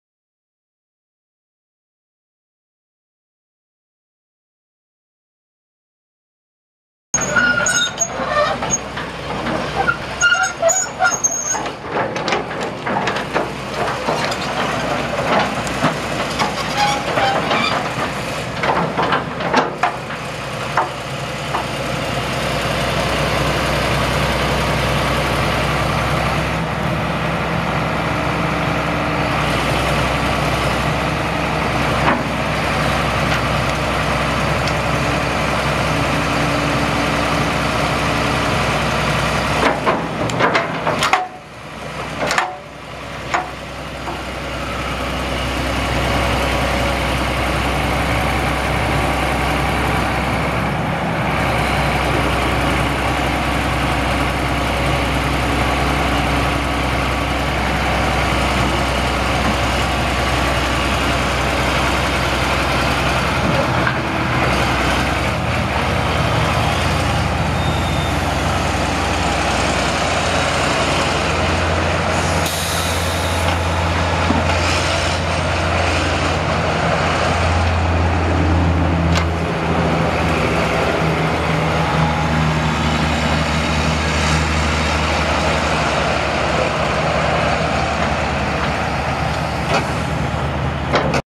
1999 John Deere 330LC excavator for sale | no-reserve Internet auction May 23, 2017
1999 John Deere 330LC excavator
11,384 hours on meter
Six cylinder turbo diesel engine
Hydrostatic transmission
Two speed travel
Enclosed cab
Heat
AM/FM
Suspension seat
Front work lights
Wipers
Standard boom
JRB hydraulic quick coupler
52"W bucket
(5) pin-on teeth
One tooth missing
32"W steel triple grouser tracks pads
Cab damage
Serial FF0330X080200
Unit# 63007